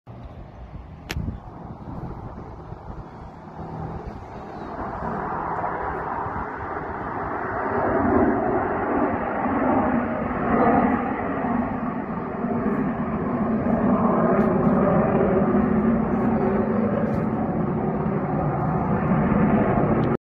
February 19, 2024
Blue Angel's practice at Pensacola Florida sorry from cell phone and I could not see my cell screen in the sun ( language)